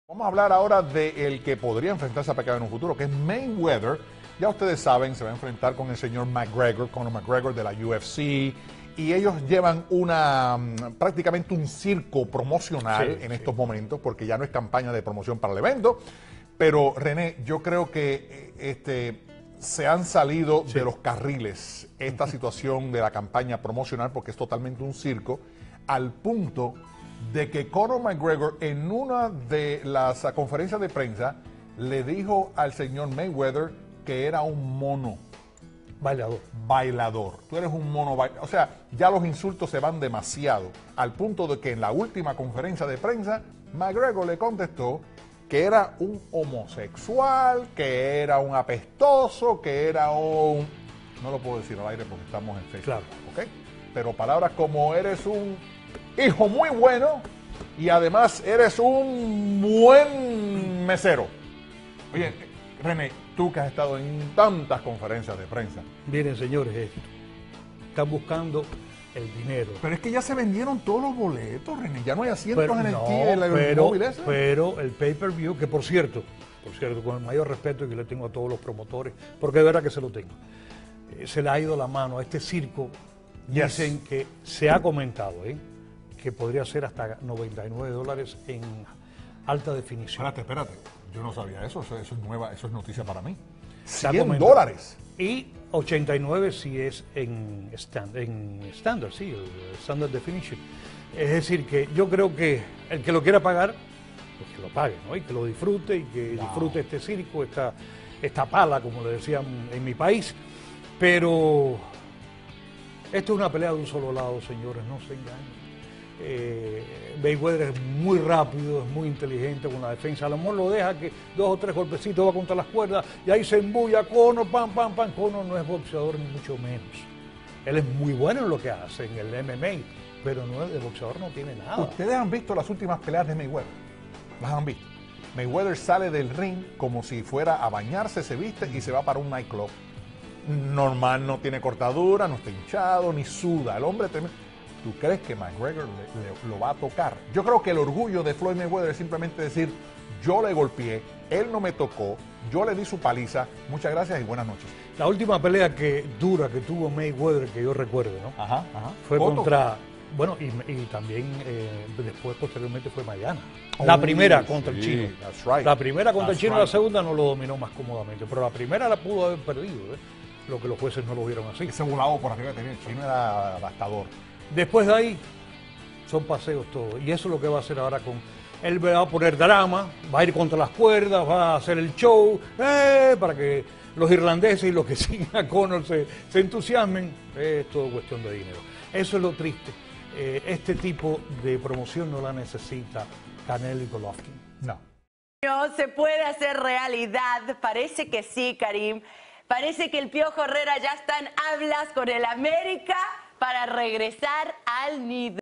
Mayweather vs McGregor, pierde sentido promocional | Más Deportes | Telemundo Deportes
Video oficial de Telemundo Deportes. Nuestros expertos analizan la gira de promoción que han protagonizado el boxeador estadounidense y la estrella de la UFC. ¡Checa los detalles!